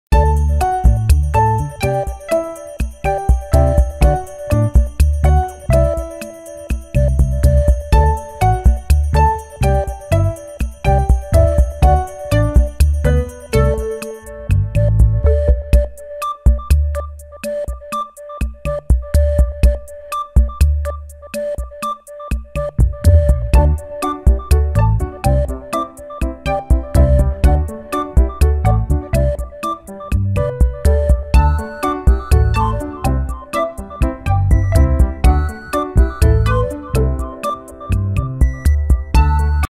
Add "Dr" to your name with Professional Doctorate program by East Bridge College
Professional Doctorates are advanced degree programs that combine academic study with professional practice.

Some of the advantages of a Professional Doctorate include:

🏆The ability to apply academic knowledge to real-world professional practice: Professional Doctorates are designed to bridge the gap between academia and professional practice

🏆Opportunities for career advancement: A Professional Doctorate can open doors to new career opportunities, such as leadership roles in industry or academia.

🏆Development of expertise in a specific field: Professional Doctorates allow students to delve deep into a specific area of study and become experts in their field.

🏆Flexibility: Professional Doctorates are often offered in a flexible format, such as online , making them accessible to working professionals.

🏆Networking opportunities: Professional Doctorates often bring together students from diverse backgrounds and industries, providing opportunities to build a professional network.